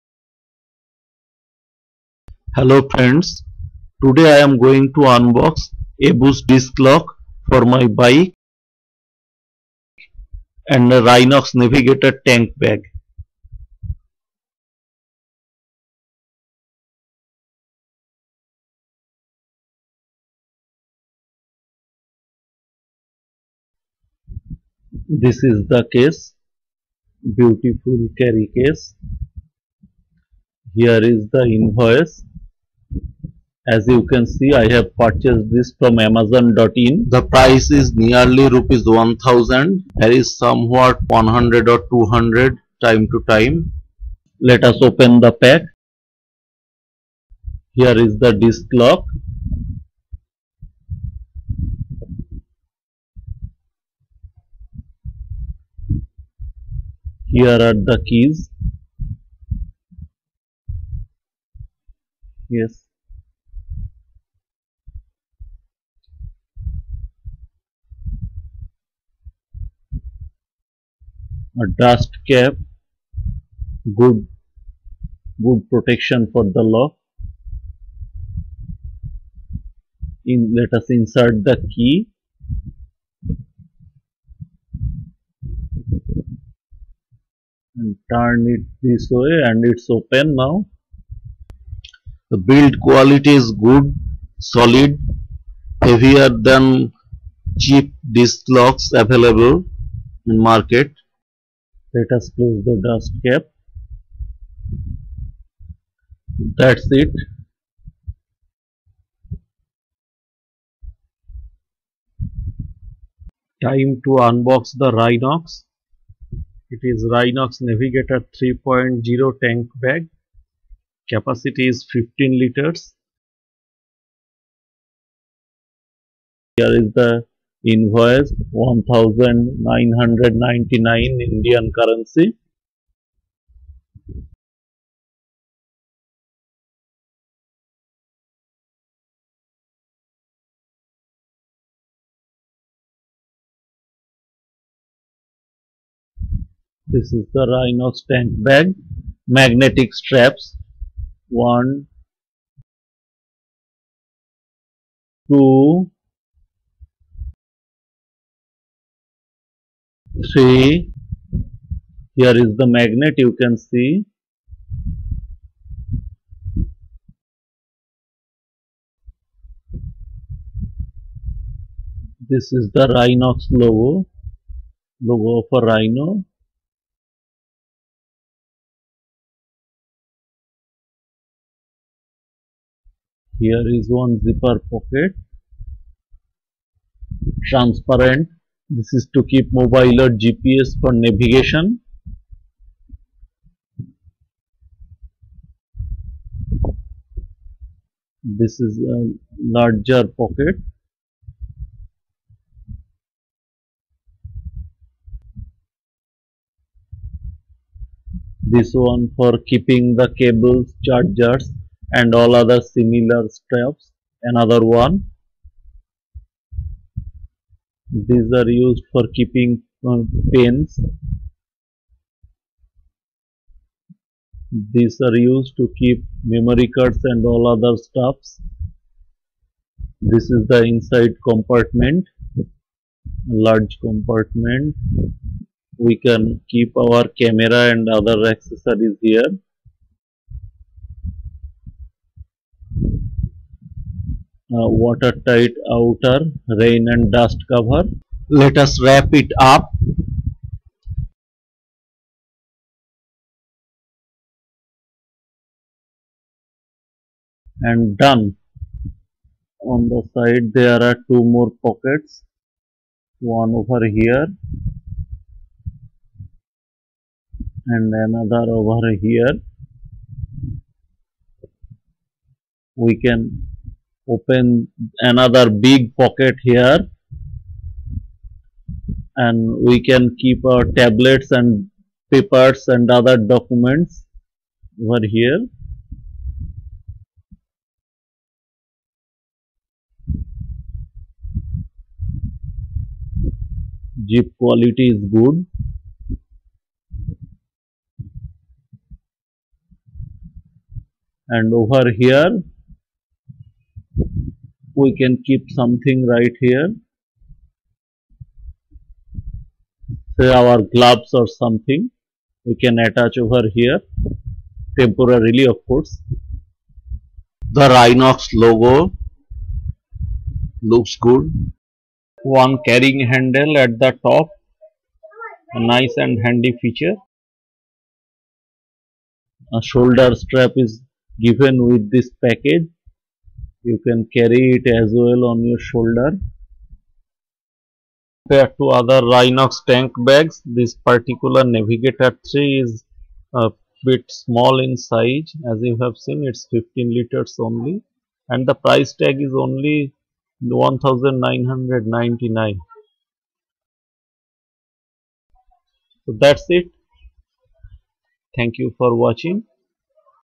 DISC LOCK AND TANK BAG
I had purchesed one Abus disc lock. It looks solid, better than the cheap disc locks. I also ordered a Rynox navigator tank bag. It is 15 lit.in size. But since I want to ride as light as I can, I choose this one.By the way, no one is paying me for this vdo. I only want to share my experience with fellow riders.